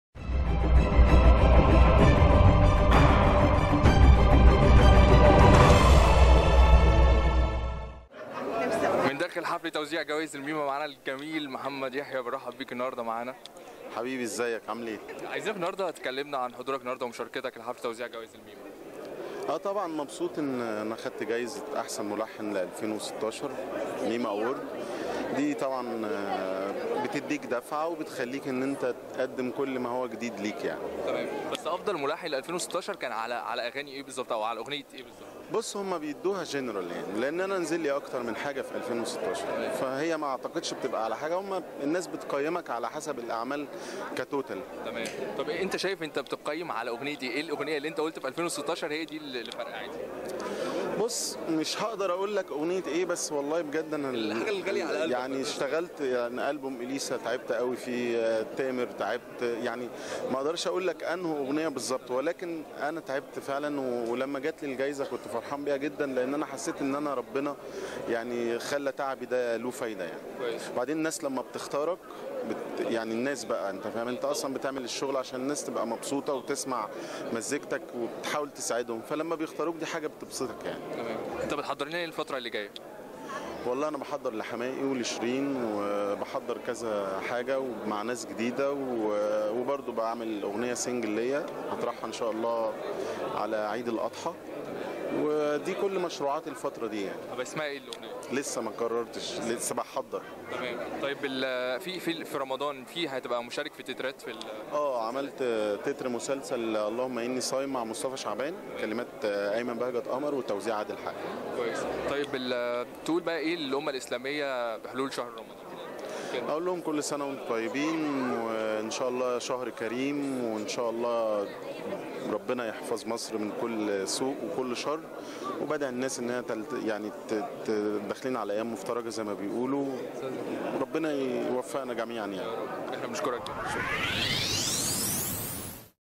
أخبار اليوم | جوائز "الميما ".. محمد يحيى أفضل ملحن لعام 2016
فريق عمل أخبار اليوم TV:
رئيس تحرير بوابة أخبار اليوم: الكاتب الصحفي علاء عبد الهادى